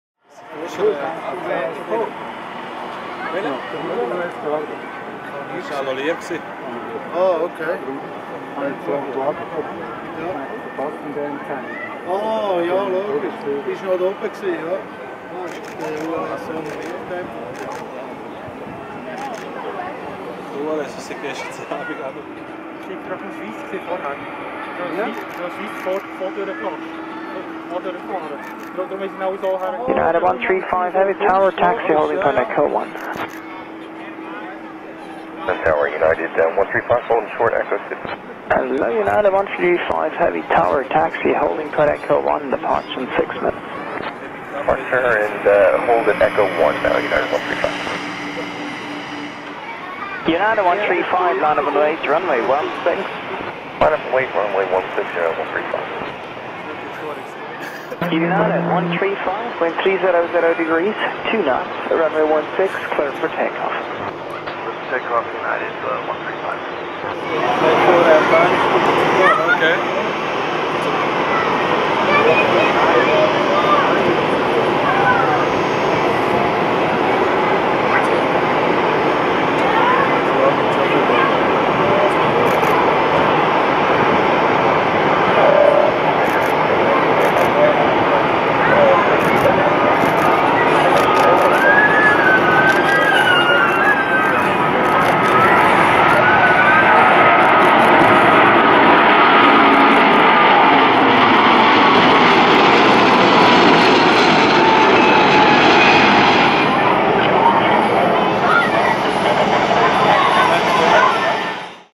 United Airlines B777-222 [N769UA] take off in Zurich (ZRH/LSZH) [Full HD]
As of this date and for the summer season, UA upgrades it’s two daily flights with 777 instead of 767. Sorry for the hot waves at 00:51, but it was very hot this day (about 30° celsius !).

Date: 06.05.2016
Operator: United Airlines
Plane: B777-222
Flight number: UA135
Take off: Runway 16
Origin: Zurich Airport (ZRH / LSZH)
Destination: New York Newark Liberty Intl Airport (EWR/ KEWR)
Registration: N769UA
Delivery: 1995

(I used LiveATC.net)